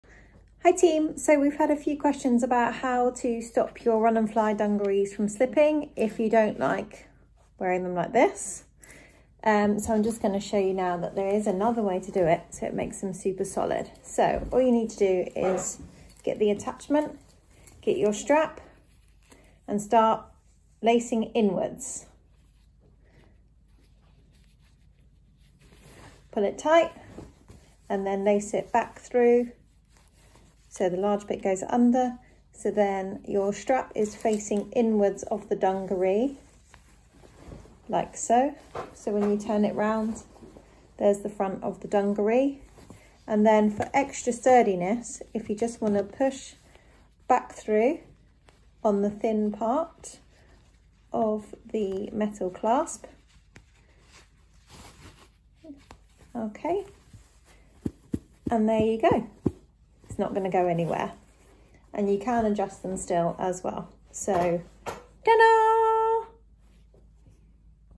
How to stop your dungarees from slipping in less than a minute
Fed up of your straps always falling down? Well watch this one minute hack to stop it ever happening again!